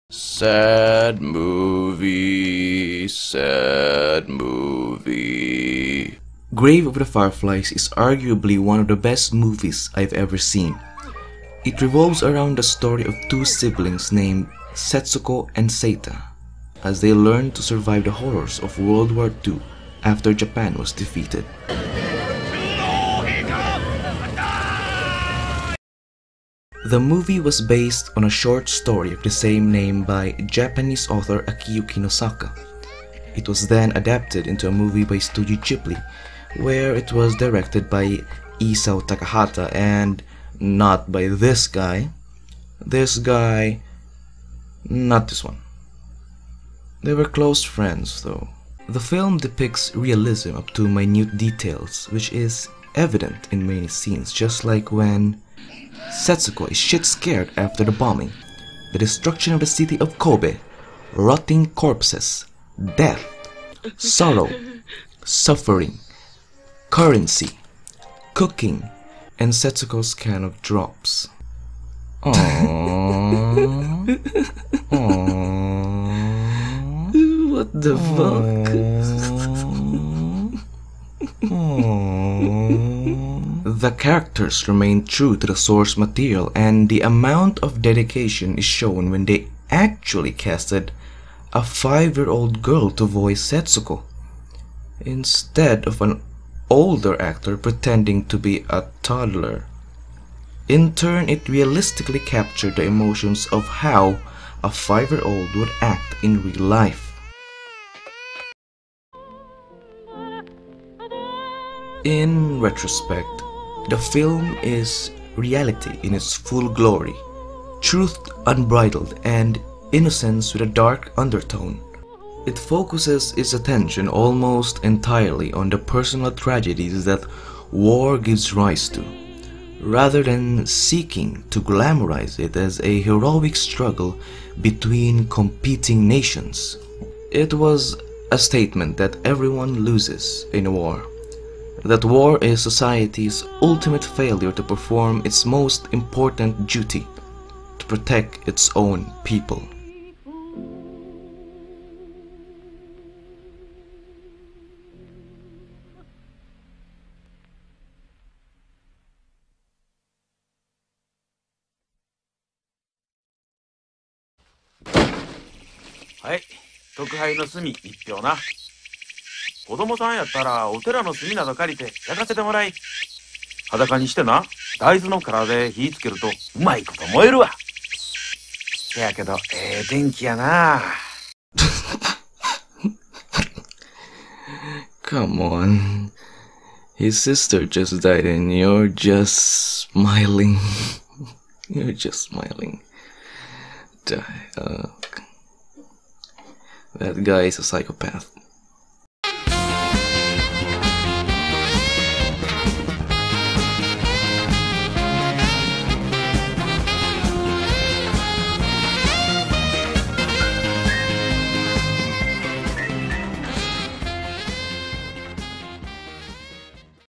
Grave of the Fireflies movie REVIEW (Just for Fun)
Reviewing the movie Grave of The fireflies in a fun dark way.